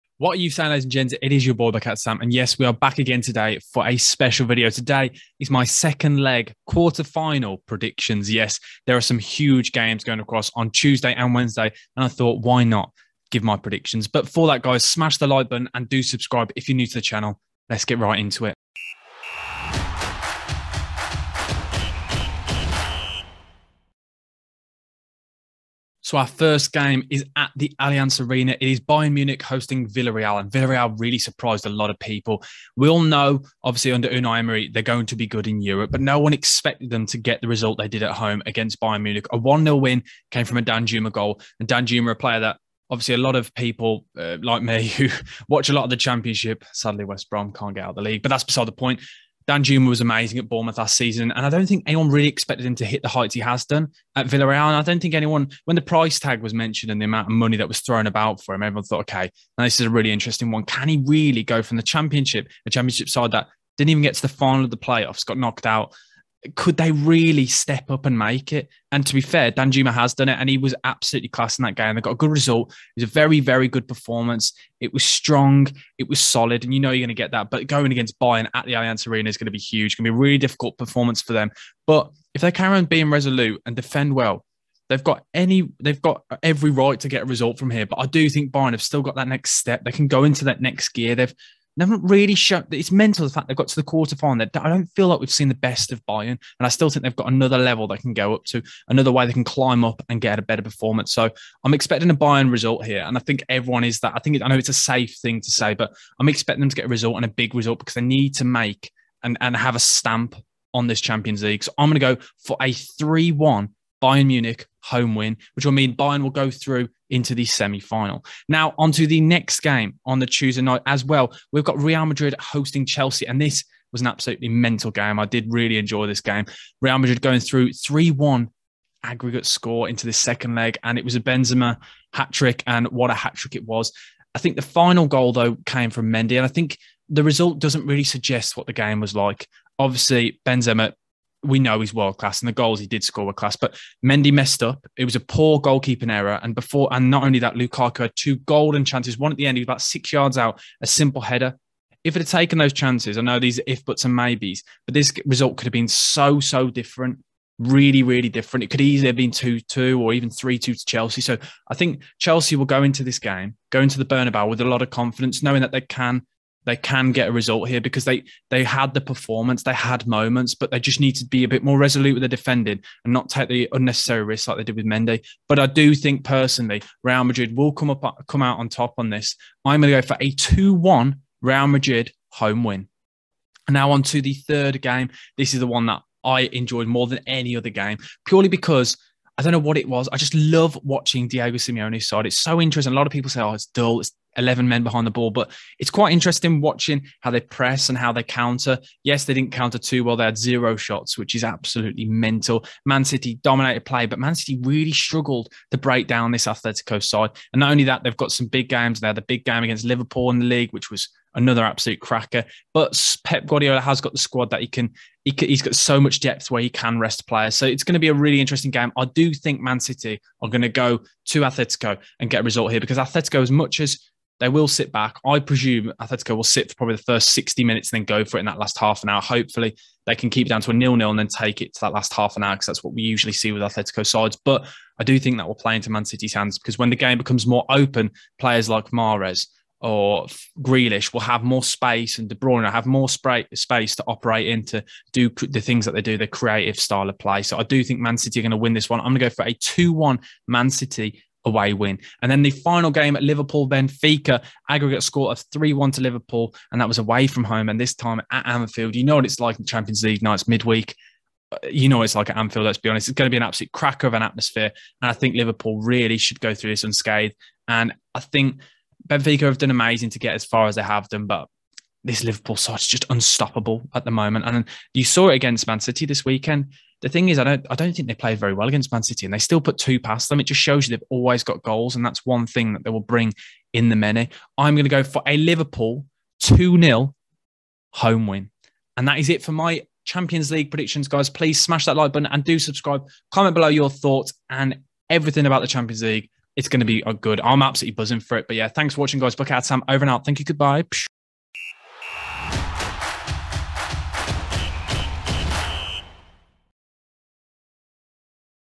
QUARTER FINAL CHAMPIONS LEAGUE 2ND LEG PREDICTIONS - ft Real Madrid vs Chelsea
Here are my predictions for the UCL Quarter final 2nd leg predictions!

Games: Real Madrid vs Chelsea, Liverpool vs Benfica, Athletico Madrid vs Man City, and Bayern Munich vs Villarreal!